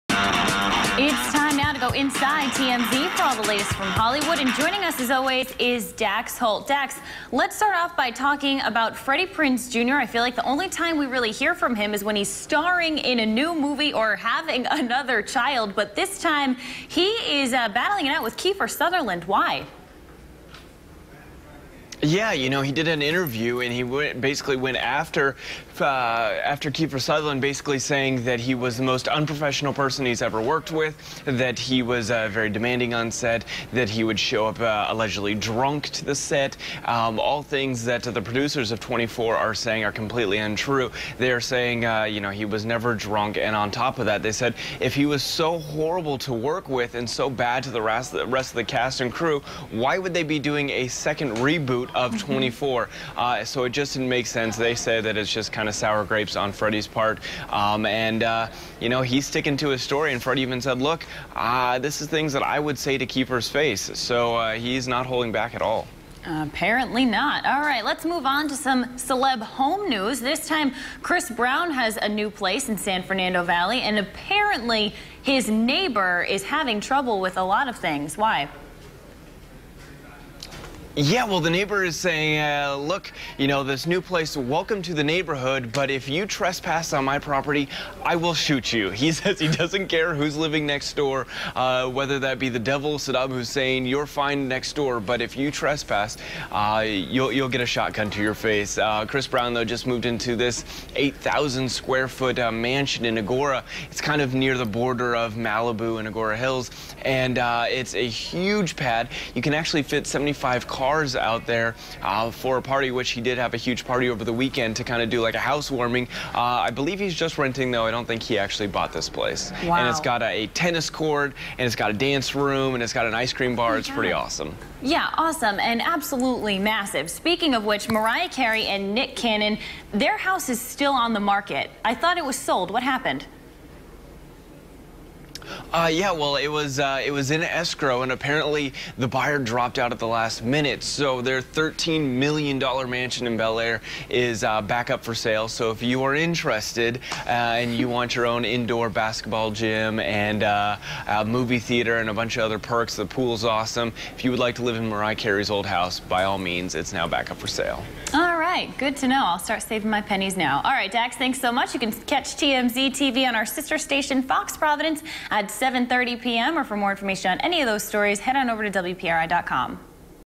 Freddie Prinze Jr starts 24 drama
Freddie Prinze Jr claimed Kiefer Sutherland has terrible to work with and often showed up to the set of 24 drunk.